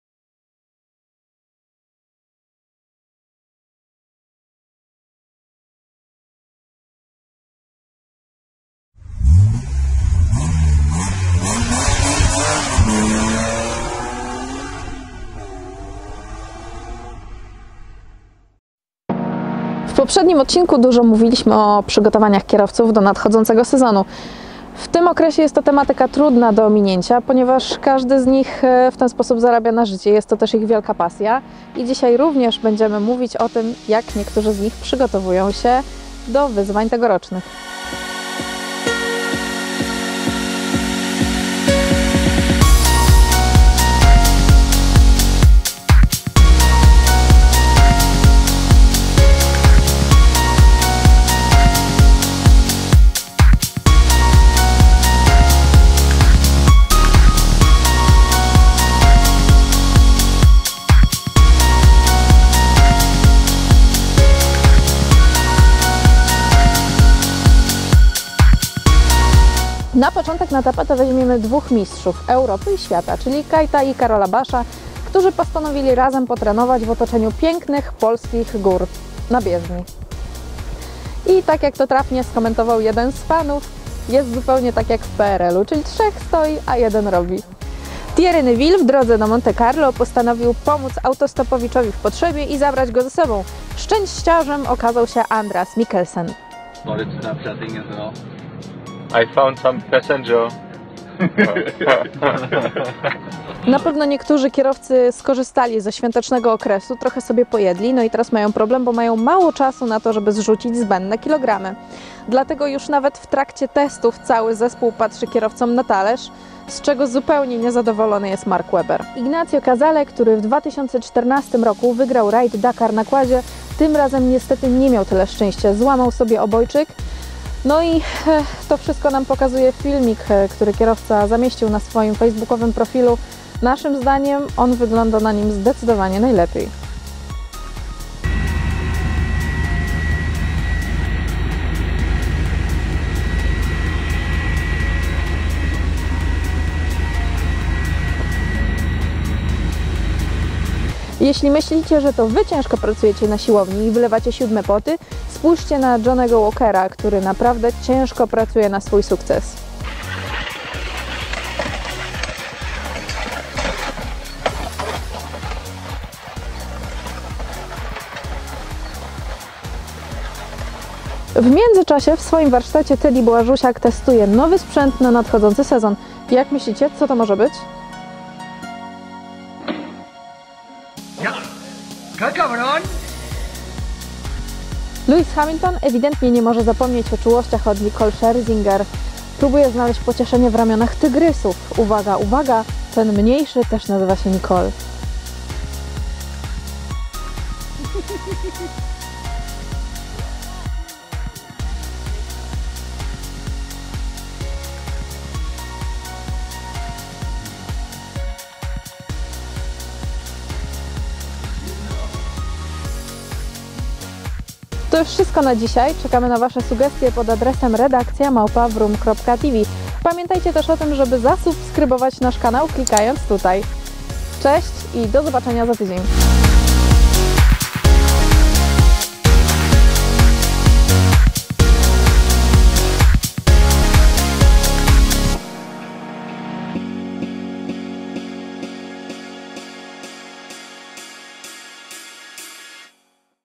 WrOOOOm Tv - Plotki ze świata motorsportu #3
W tym odcinku Idalia Czarnocka przygląda się temu, do kogo przytula się Lewis Hamilton, sprawdza na czym jeździ Taddy Błażusiak oraz opowiada o Johnym walkerze, Thierry Neuvillu i kilku innych znanych ze swiata motorsportu!